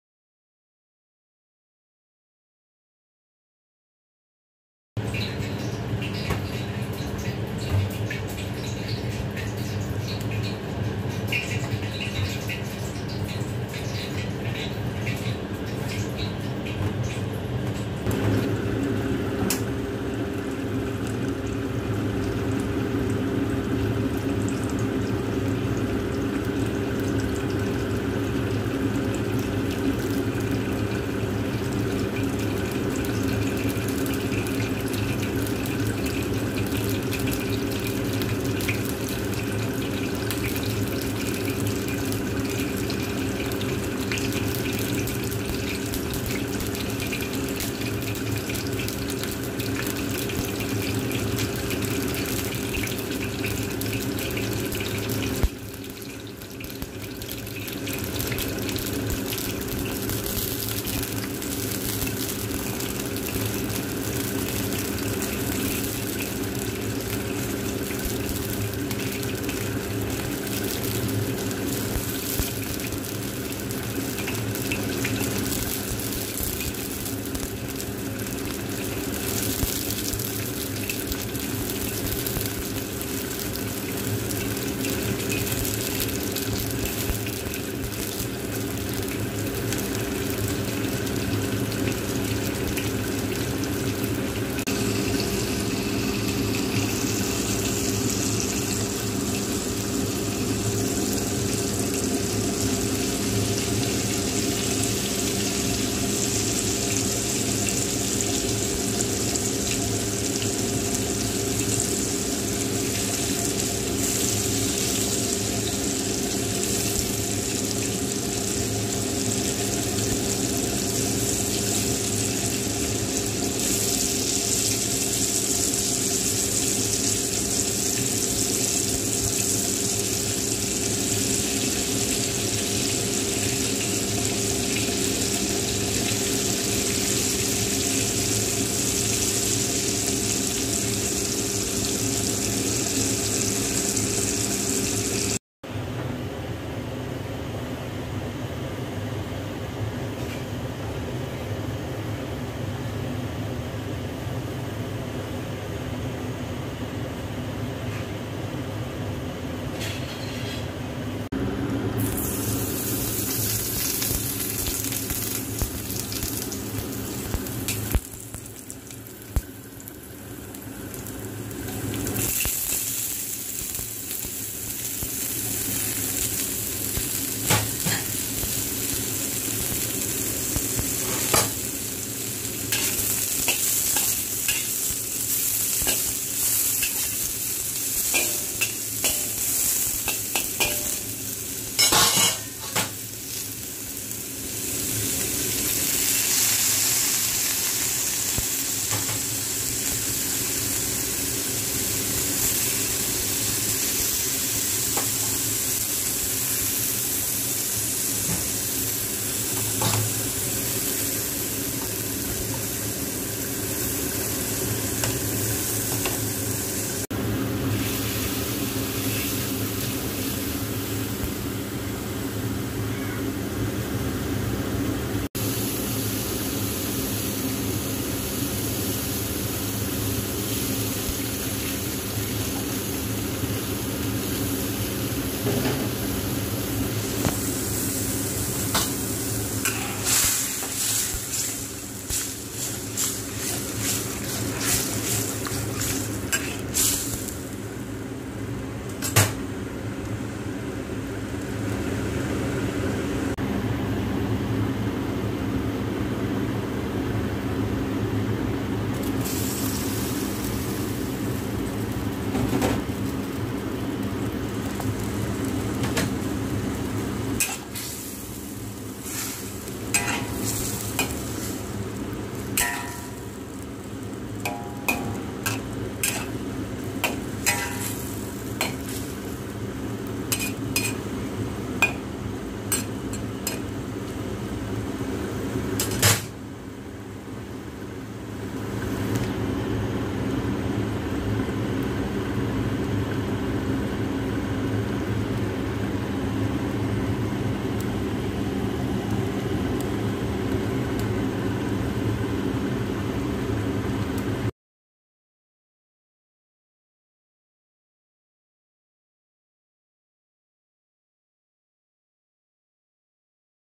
How to make Sweet And Sour Fried Chicken Fillet.
My own Homemade Chicken Fillet Sweet and Sour.